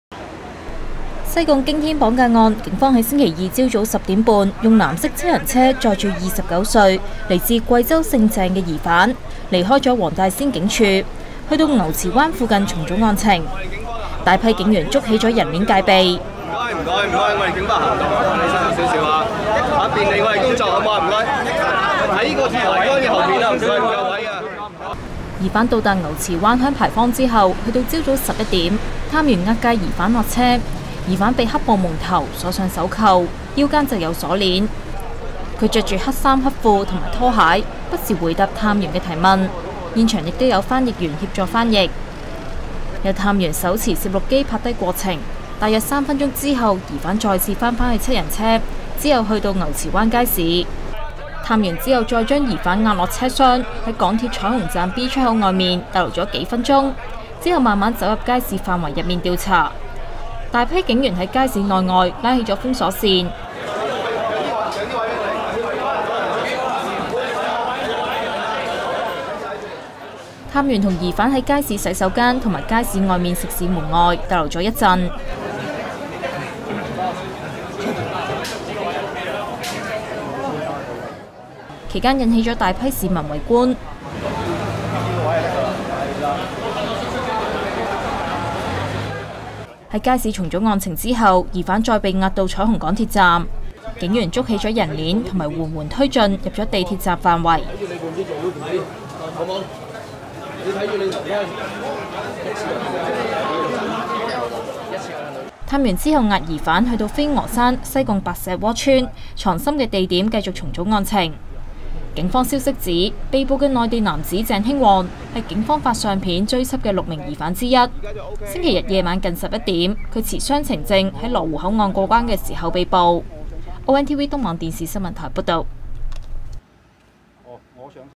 綁架案疑犯抵牛池灣重組案情
西貢驚天綁架案警方押解疑犯到牛池灣重組案情。